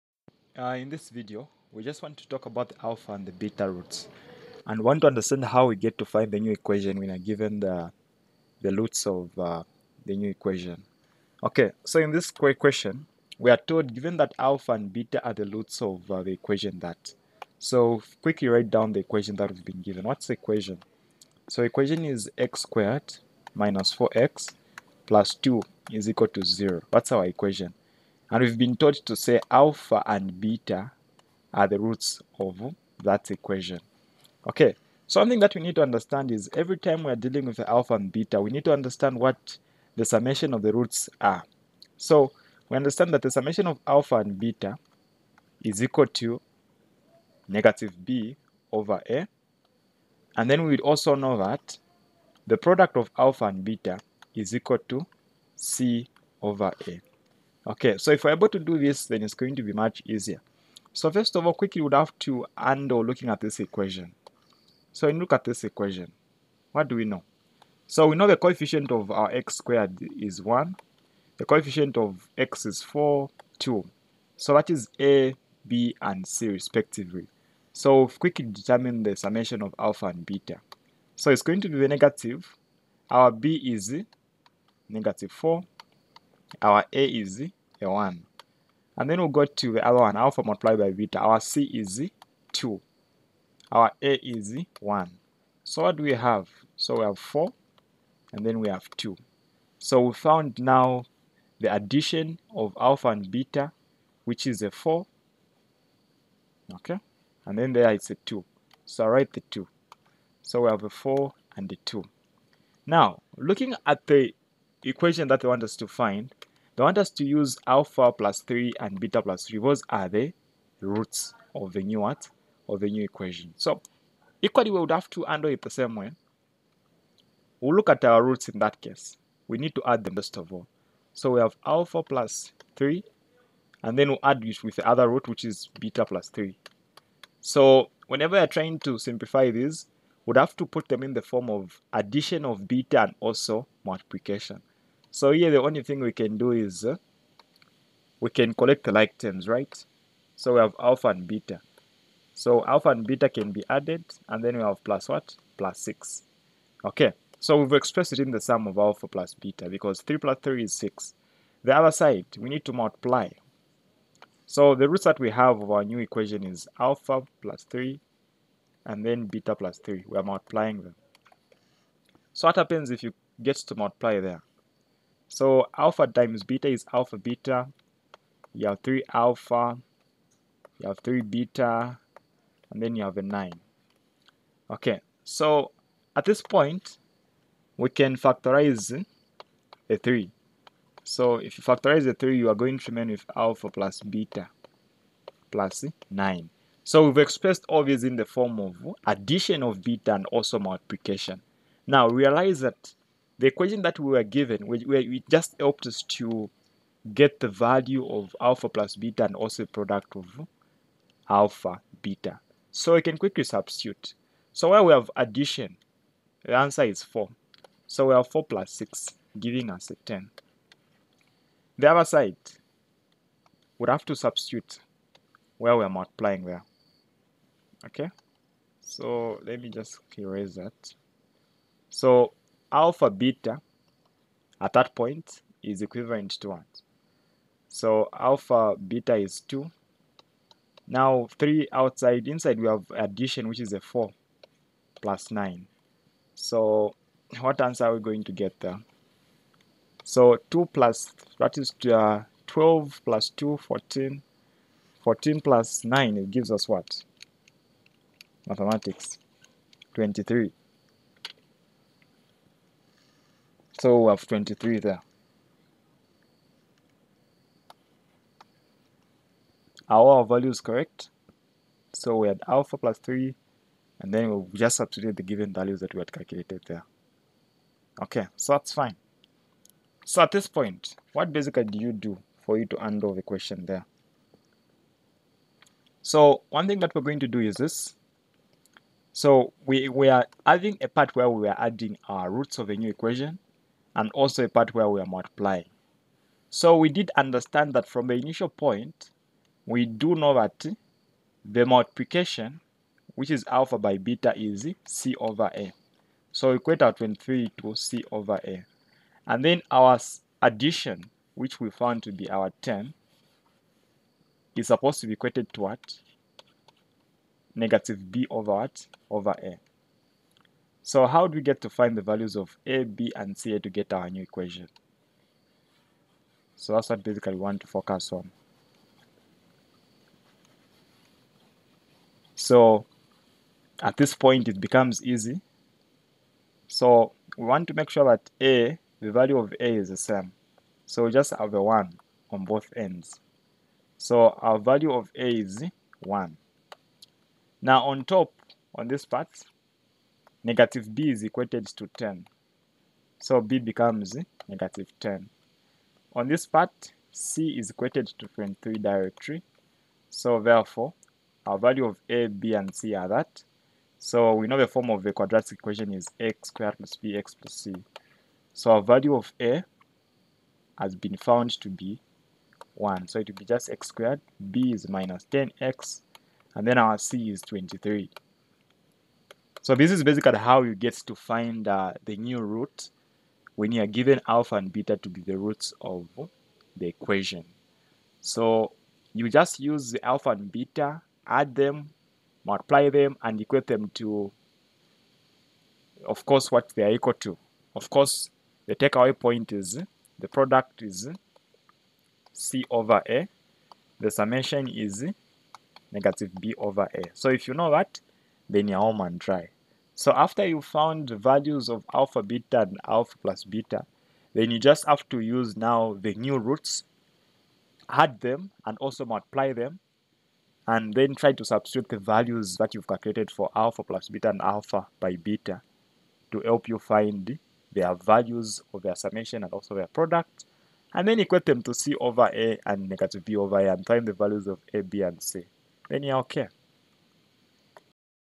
alpha and beta roots of quadratic equation | Finding new equation
In this video we learn how to use alpha and beta roots of quadratic equation to find a new equation given roots.